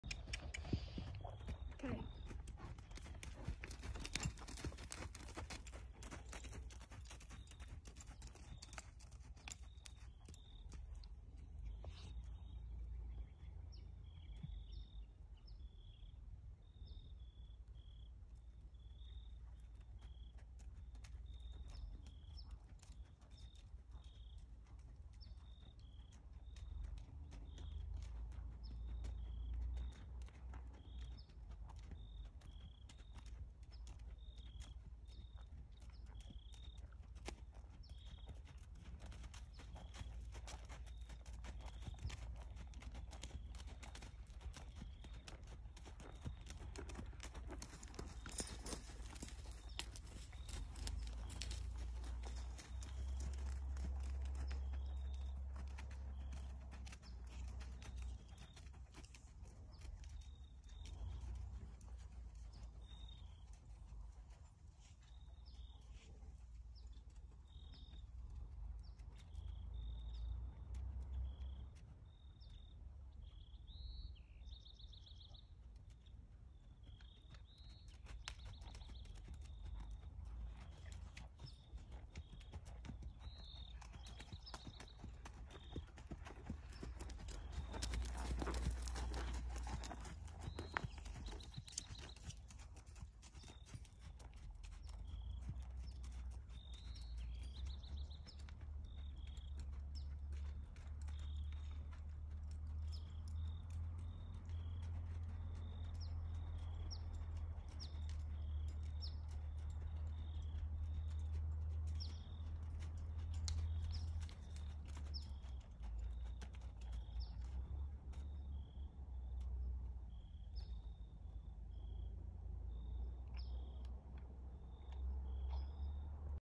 Dressage Maneuvers WalkTrot English
Debbie and Batman’s entry into the ACMC Virtual Show - April 2020